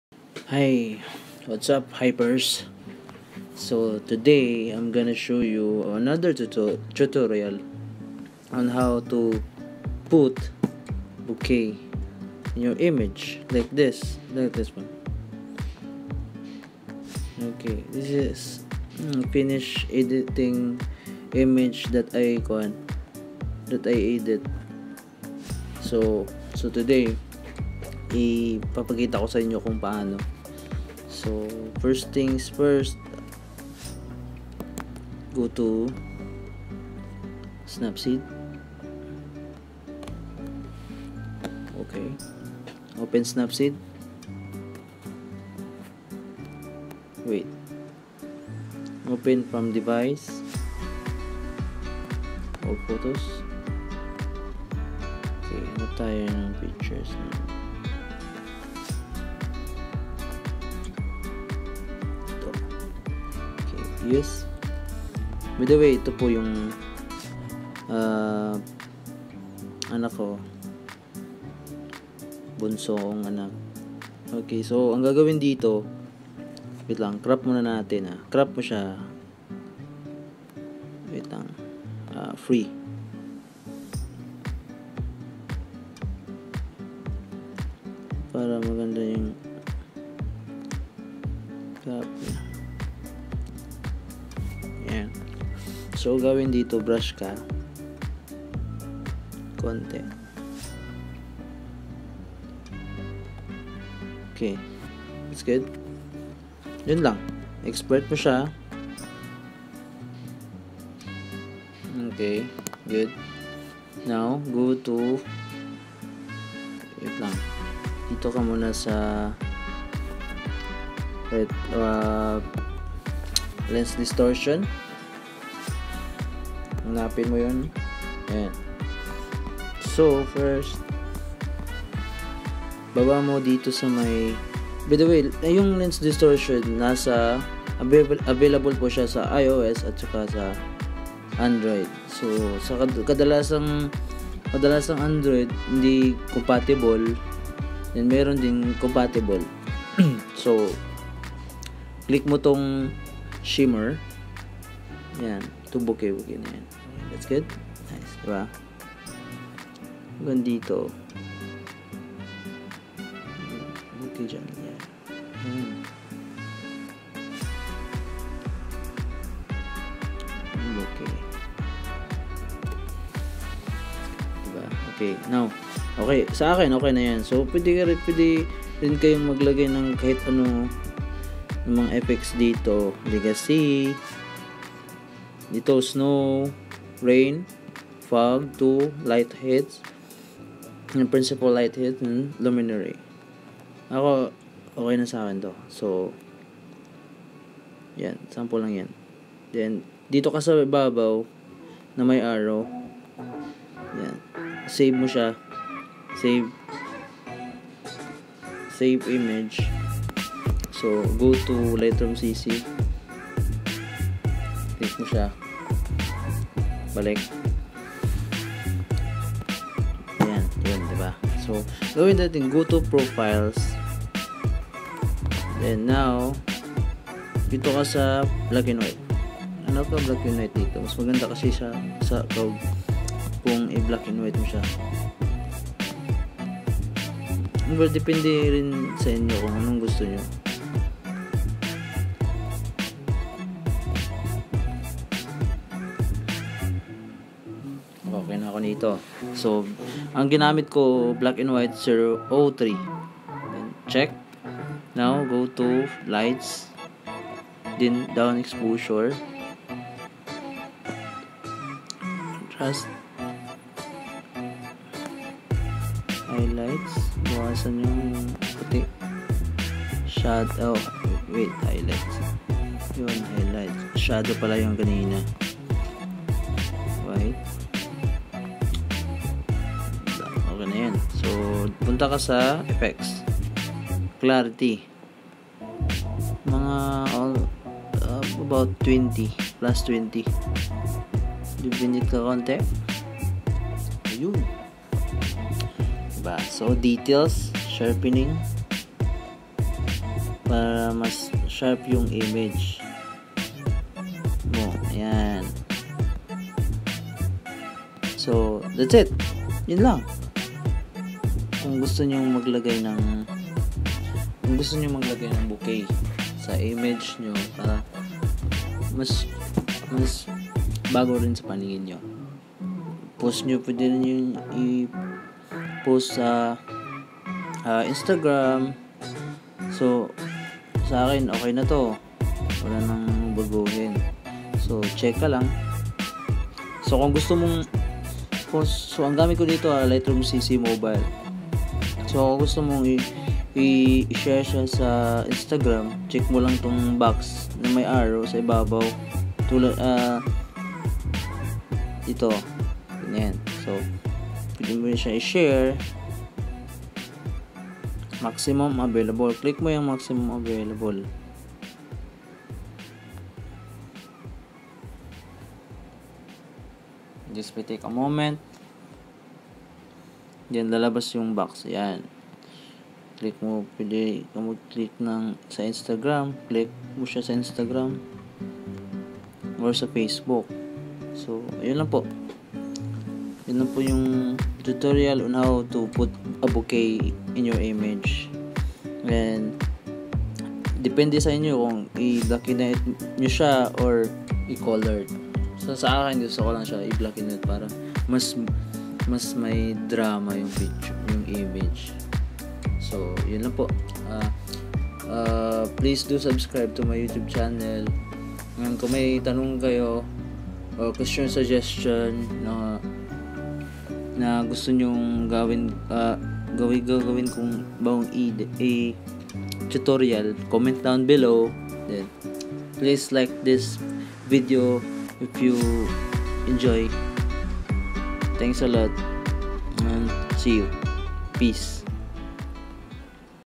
THE SIMPLE BOKEH EFFECT USING 3 EDITING APPS
HI GUYS TODAY'S TUTORIAL IM GONNA SHOW AGAIN THE BOKEH EFFECT IN LENS DISTORTION FOLLOW MY VIDEO ON HOW I DO IT!

(TAGALOG VERS.)
PASINSYA NA SA TAGALOG KO NAG AADJUST PALANG HEHE :-)
HINDE SANAY EH! PRO TRY KO PARIN ANG BEST KO PARA MAIINTINDIHAN NYO.SALAMAT PO.:-) GODS SPEED

shout out! Non-Copyrights music Vlog for their amazing music.